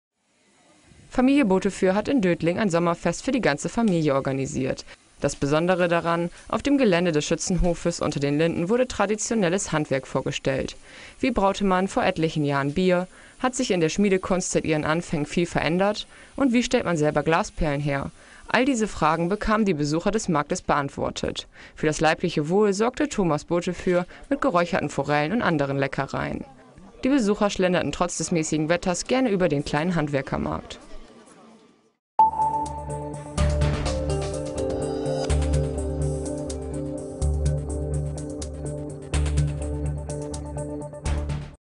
Dötlingen: Sommerfest stellt Handwerk vor
Dötlingen, 16. August 2015: Familie Botefür hat in Dötlingen ein Sommerfest für die ganze Familie organisiert. Das Besondere daran: Auf dem Gelände des Schützenhofes Unter den Linden" wurde traditionelles Handwerk vorgestellt."